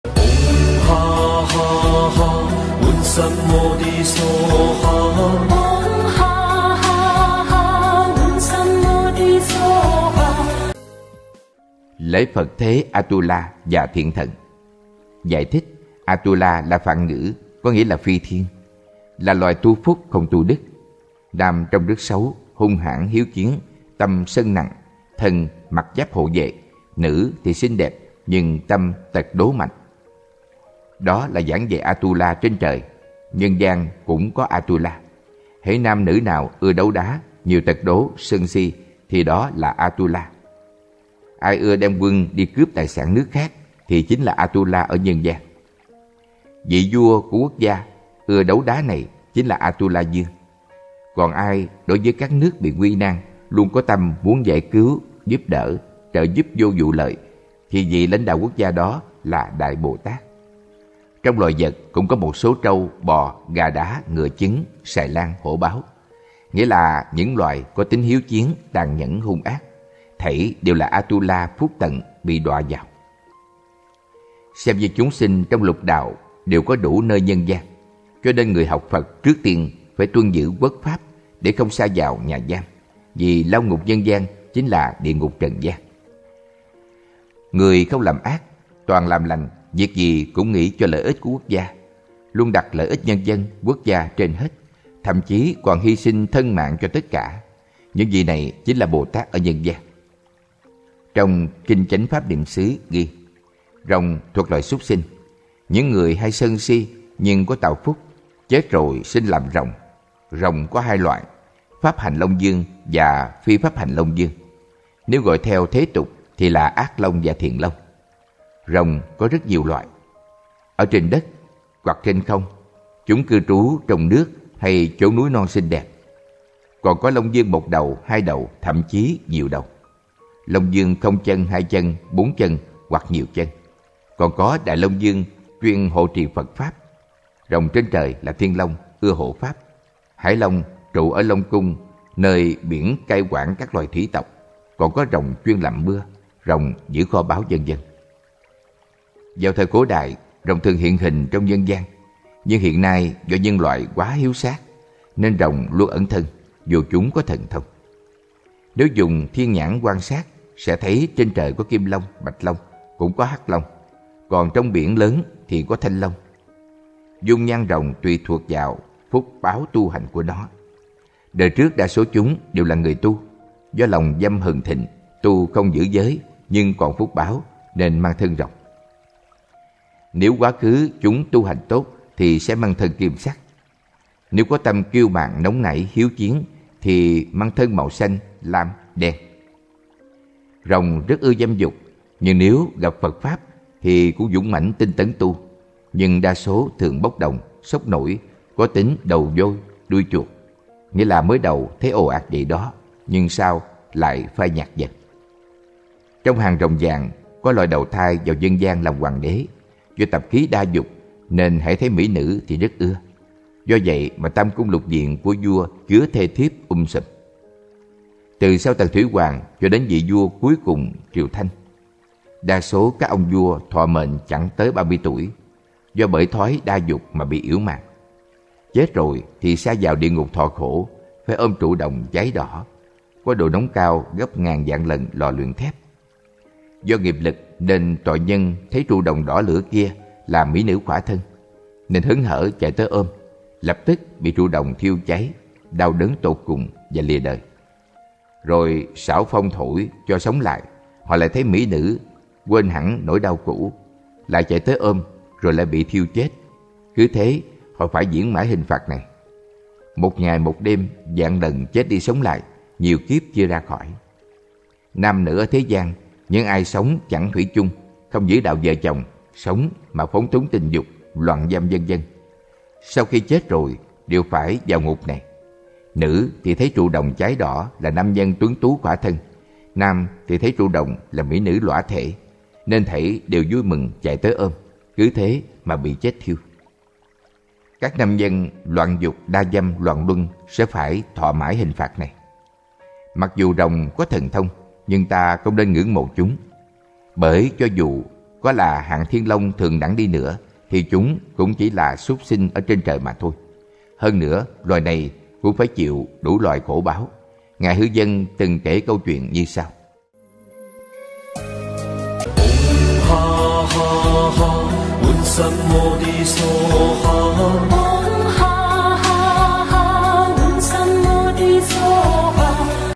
Lễ Phật thế A Tu La và Quỷ Thần | Hạnh Đoan
Lễ Phật thế A Tu La và Quỷ Thần
Trích Phụ giải Lương Hoàng Sám tập 2 - Hạnh Đoan
Báo Ứng Hiện Đời Tập 5 (Phụ giải Lương Hoàng Sám)